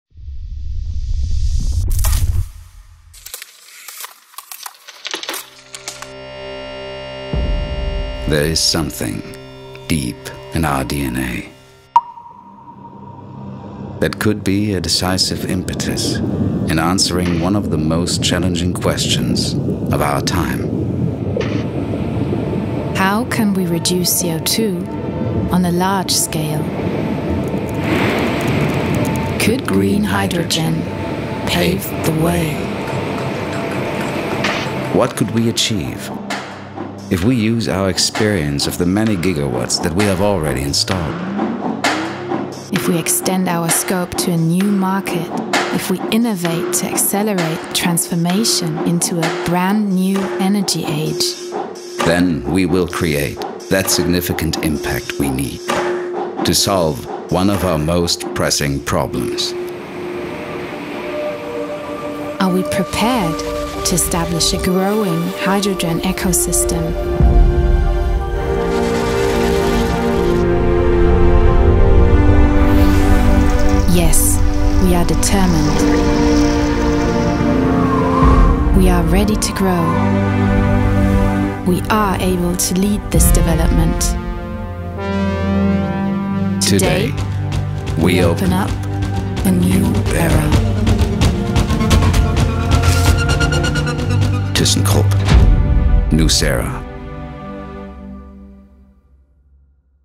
thyssenkrupp nucera - brand launch
„thyssenkrupp nucera" - Ein neuer Name, der für Aufbruch steht.

Großes Medien Echo für eine wirklich gute Geschichte – mit grünem Wasserstoff einen signifikanten und positiven Beitrag zum Klimawandel zu leisten. Wir sind stolz Teil dieses großen Vorhabens zu sein, denn wir durften den Namen, das Narrative und das Design zusammen mit unserem Partner deluxe und mit dem thyssenkrupp-Team entwickeln.
Ein spannendes Projekt im Co-Creation für kreative und strategische Markenentwicklung und Kommunikation. Wir freuen uns Noch mehr freuen wir uns darauf diese nun mit strategischem Storytelling und Branding in allen Dimensionen zum Leben zu erwecken.“